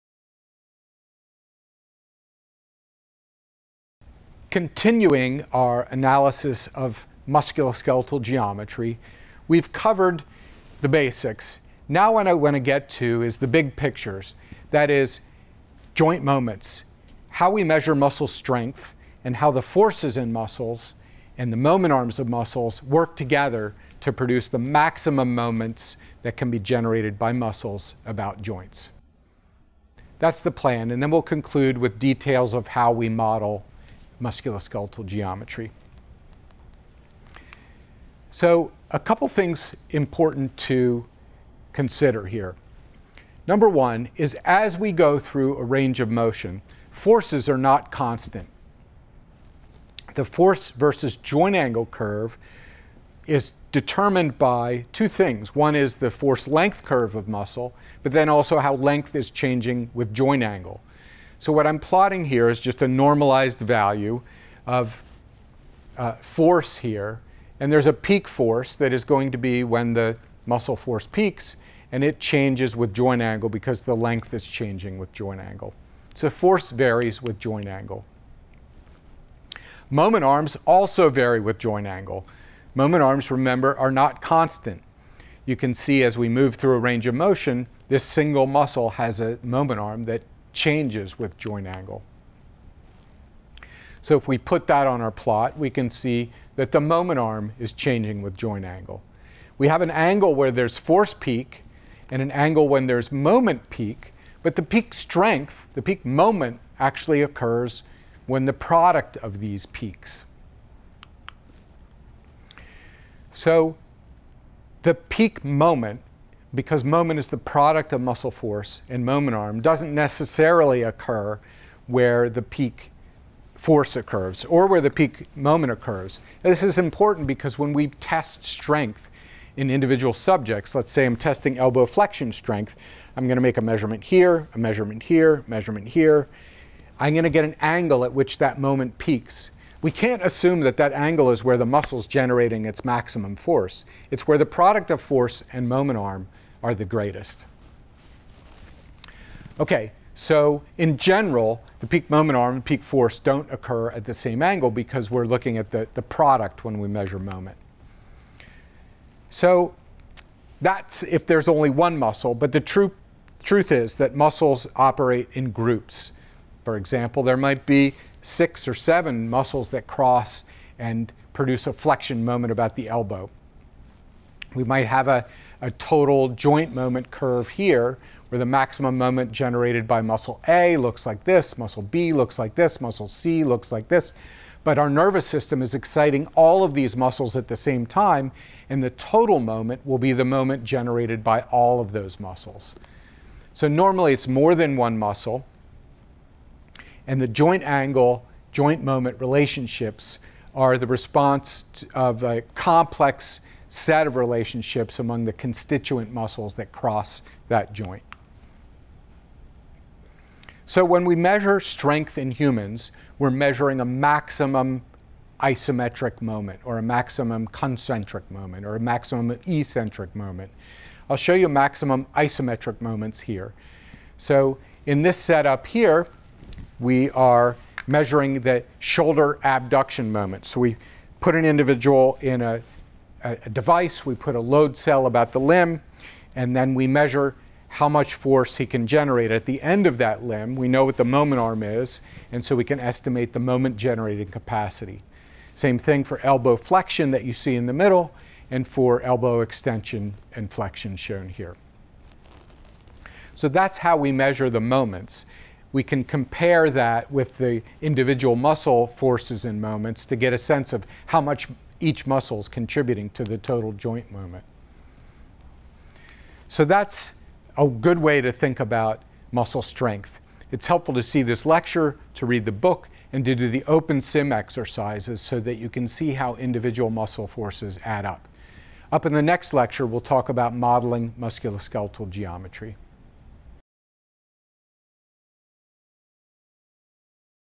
Biomechanics of Movement | Lecture 6.5: Understanding Muscle Strength
Lecture by Professor Scott Delp of Stanford University about muscle strength. Learn about joint moments, how we measure muscle strength, and how muscle forces and moment arms interact to govern the maximum moments that can be generated by muscles about joints.

This lecture covers part of "Biomechanics of Movement Chapter 6: Musculoskeletal Geometry"

Acknowledgments:
Clio Delp, Sebastian Kleppe, University of Ottawa (Video Production)
Marissa Lee, Melissa Boswell, Hannah O'Day (Content Review)
The Stanford Human Performance Lab especially Scott Uhlrich & Julie Muccini (Demos)
The University of Ottawa and Neuromuscular Biomechanics Lab of Stanford University